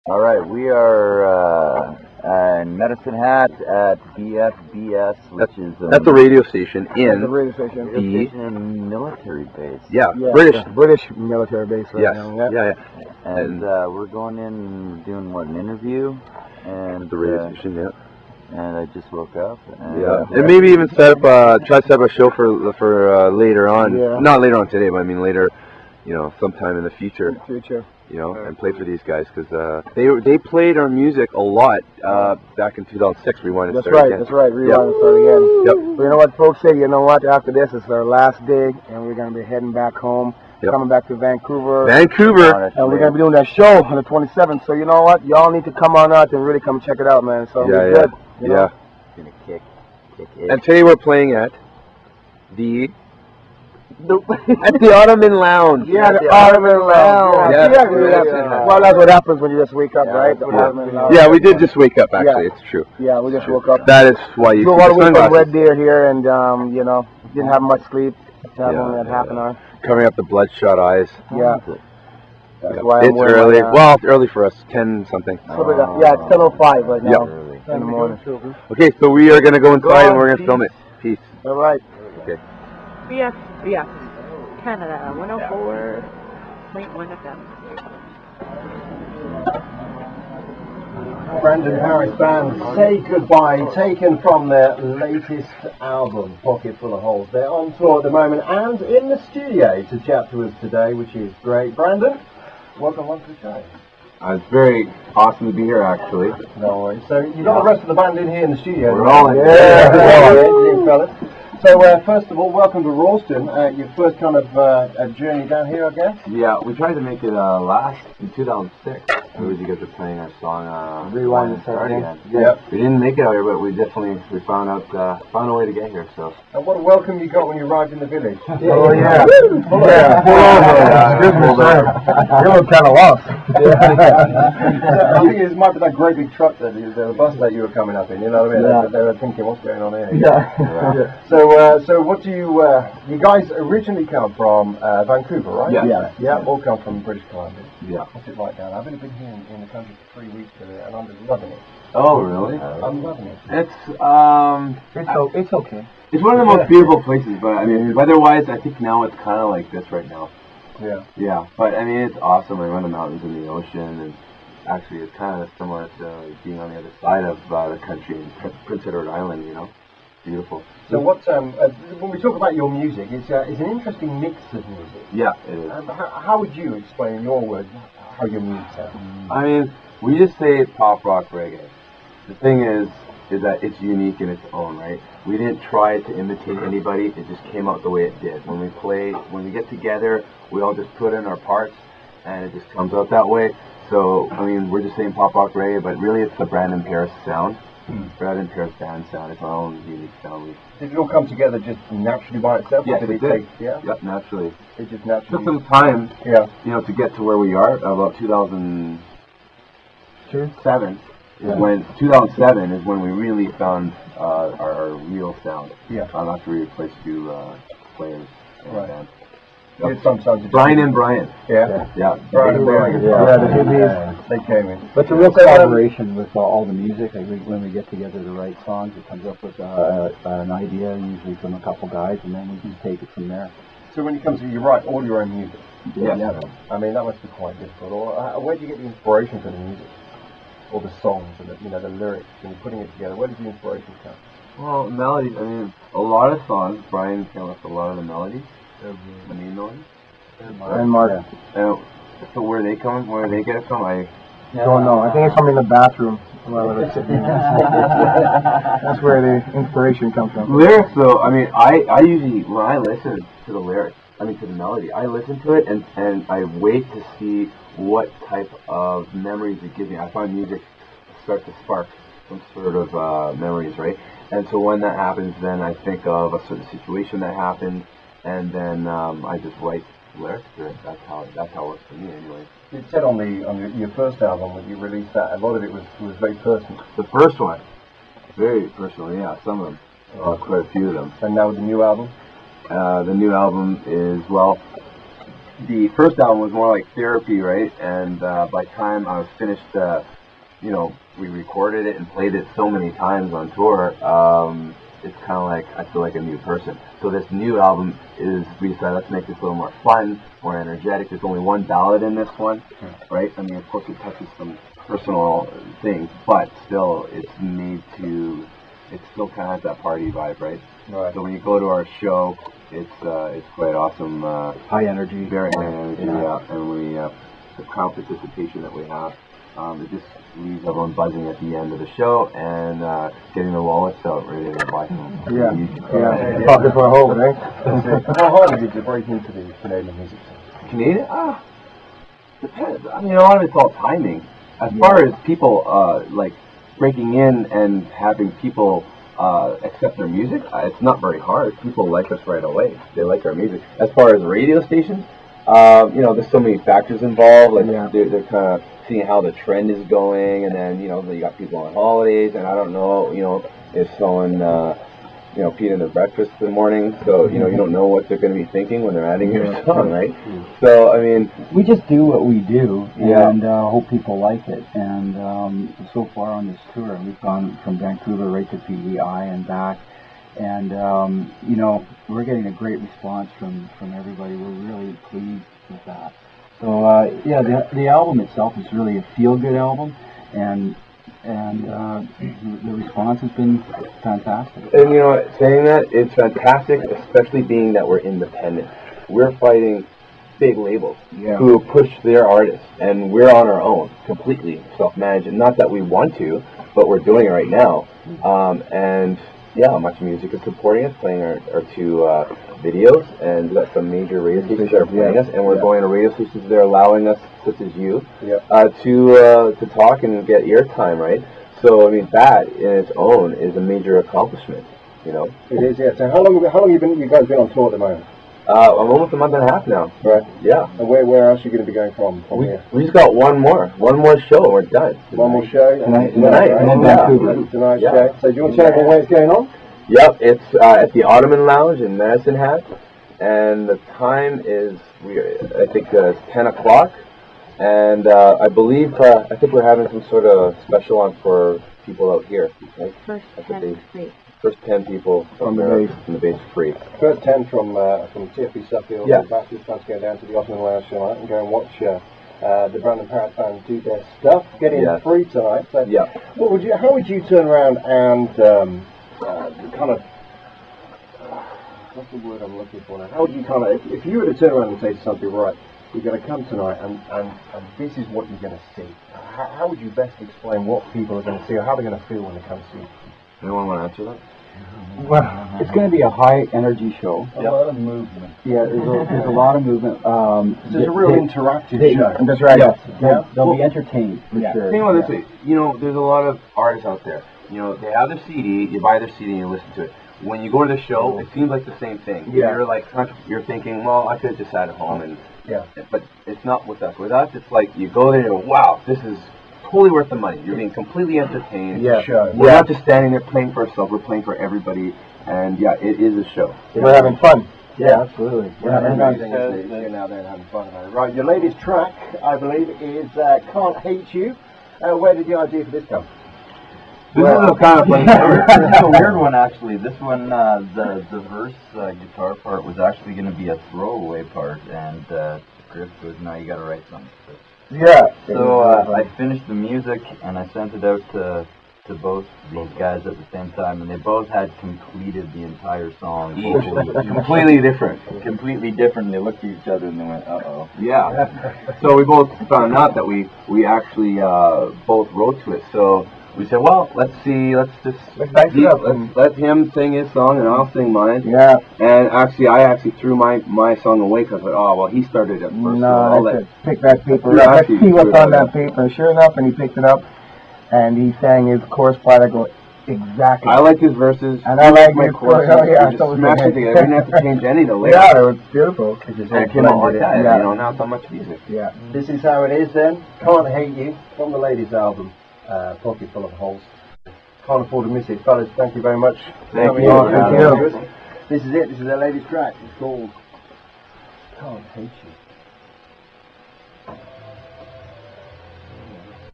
Brandon Paris Band in Medicine Hat, AB (Pocket Full Of Holes Tour) Part 1 0f 2
Brandon Paris Band heads out to BFBS 104.1FM Canada(British Forces Broadcasting Station) in Ralston, Alberta (Just outside Medicine Hat) during their "Pocket Full of Holes" Canadian Tour where they do a live interview on air!